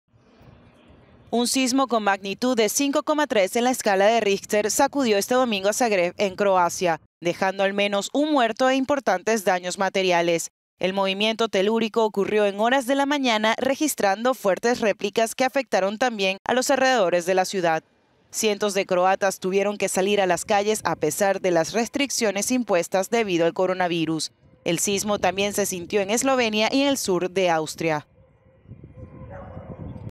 Un sismo de 5,3 importantes daños materiales en Zagreb
El temblor también se sintió en Eslovenia y en el sur de Austria. …